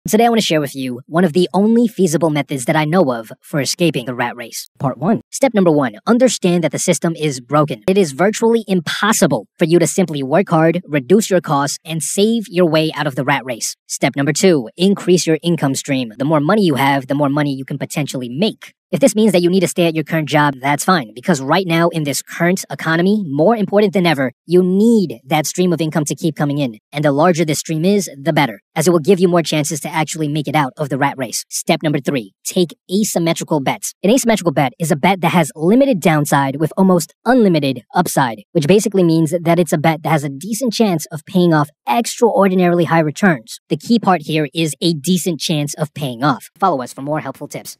How To Escape The Rat Race in 3 Steps
I want to show you how to escape the rat race. One of the only methods I know of will allow you to retire early if executed correctly.